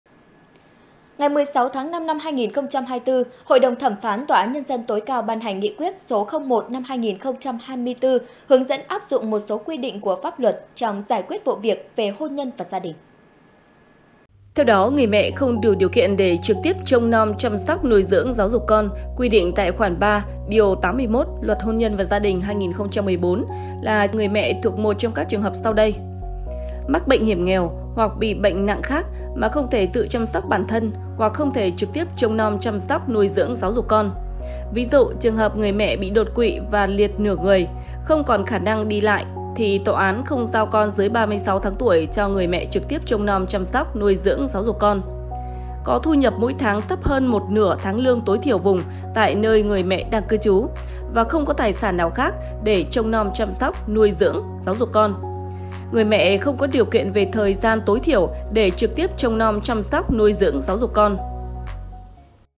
Hướng dẫn áp dụng 1 số quy định của pháp luật trong giải quyết vụ việc về hôn nhân và gia đình |THLC
Đài Phát thanh - Truyền hình Lào Cai